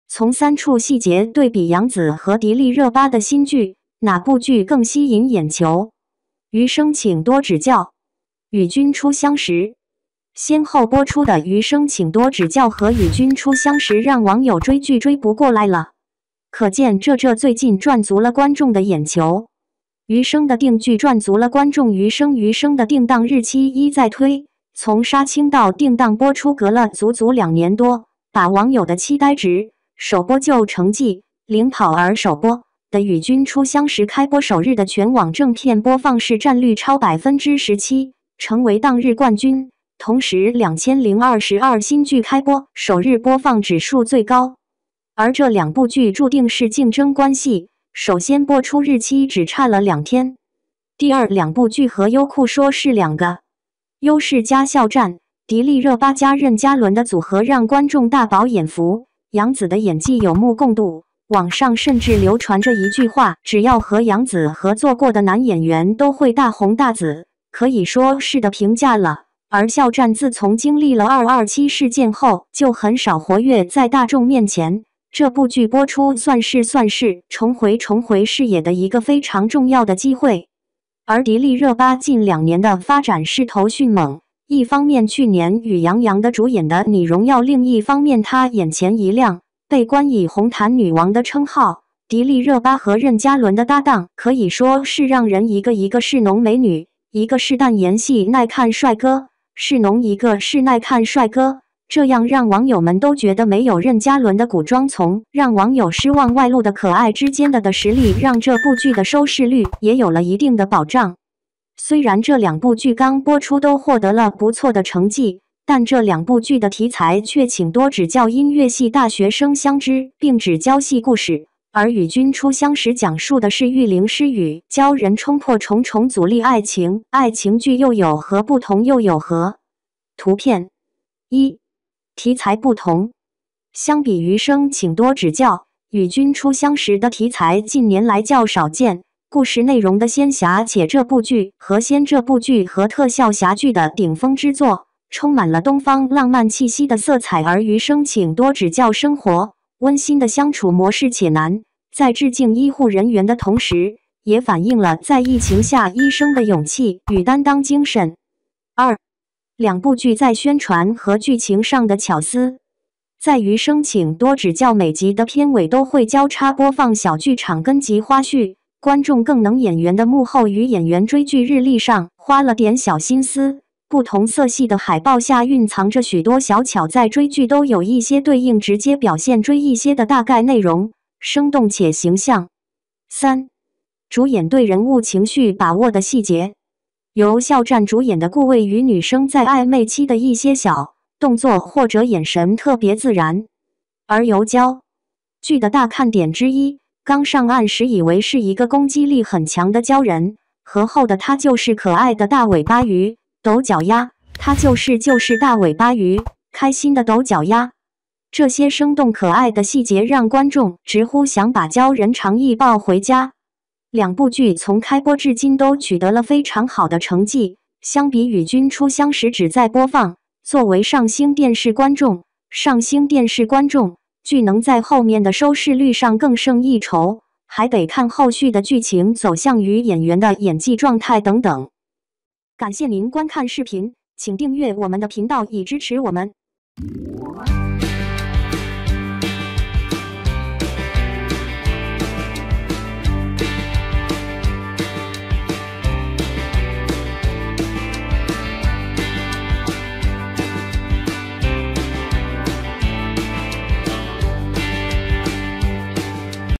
从 三 处 细节 对比 杨 紫 和 迪丽 热 巴 的 新剧 ， 哪 部 剧 更 吸引 眼球？
从 三 处 细节 对比 杨 紫 和 迪丽 热 巴 的 新剧 ， 哪 部 剧 更 吸引 眼球？  | MKN Star News.